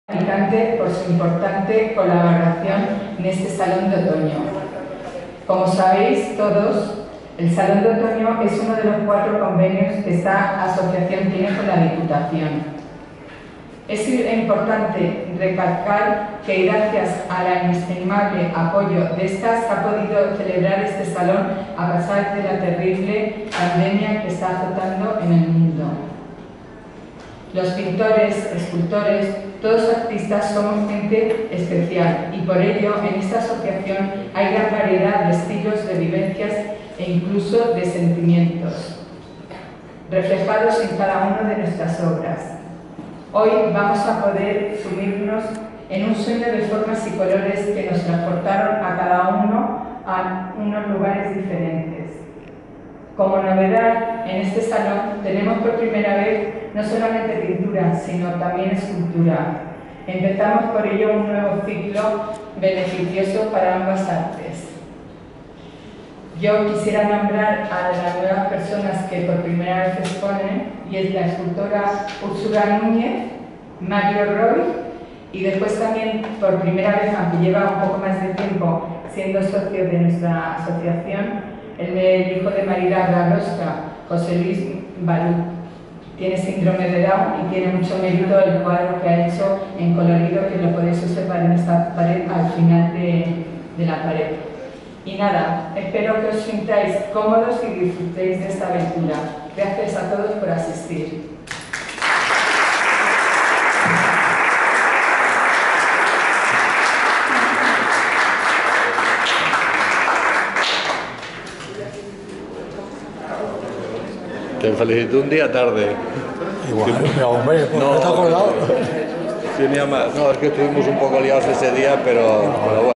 Salón de Otoño. Alicante 2020. Loles Guardiola
Hoy 3 de septiembre se ha inaugurado el Salón de Otoño en la Asociación de Aristas Alicantono por al presidentea Loles Guadiola. Escaso público por las medidas de seguridad.